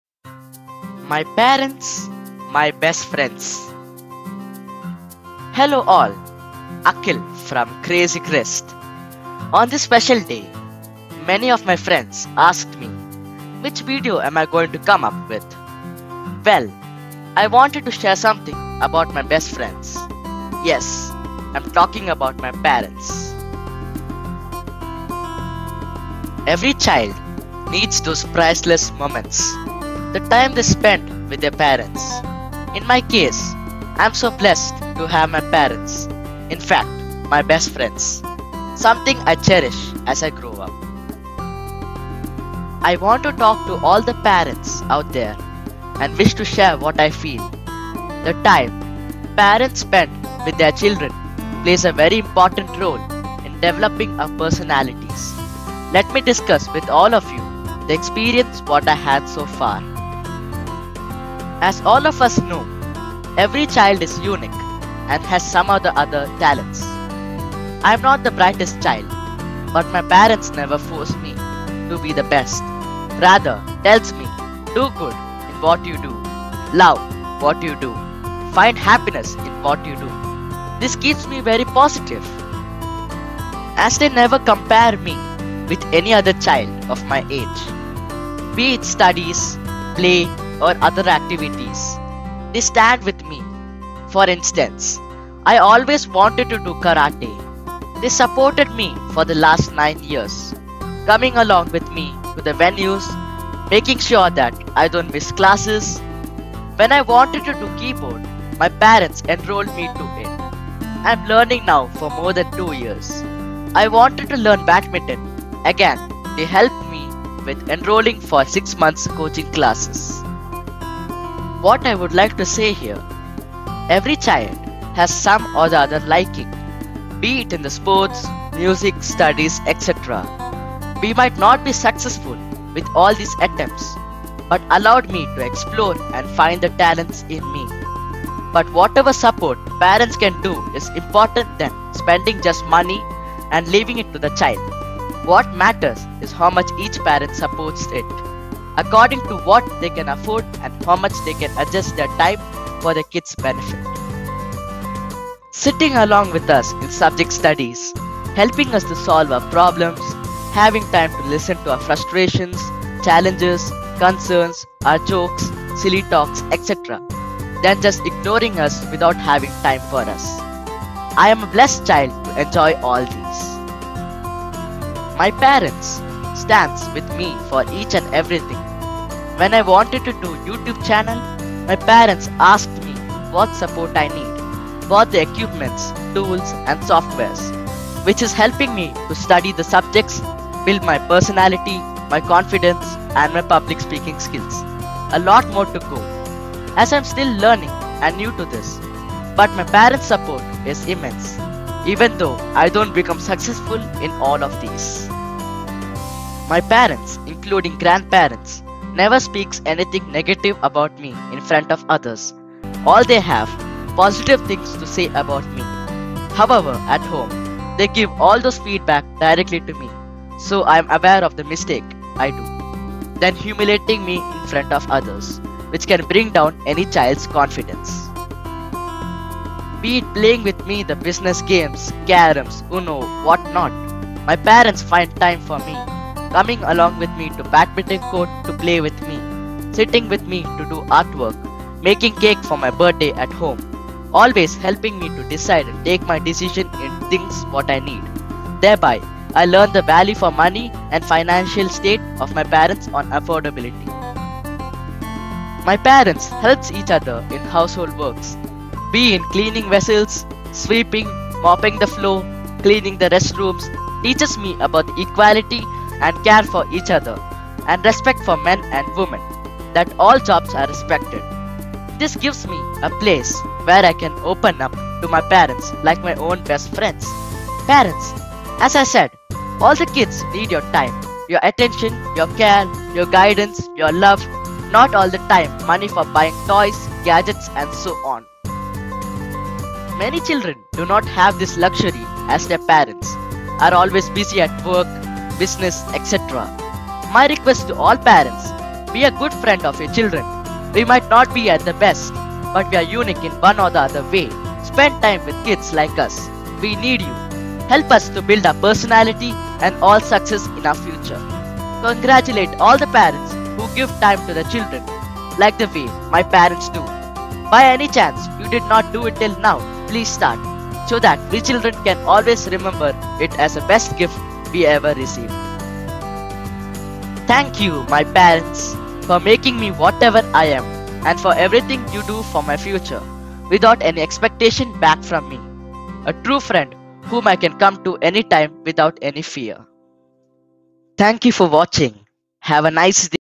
My Parents My Best Friends!
A special video dedicated to my Parents on their wedding anniversary.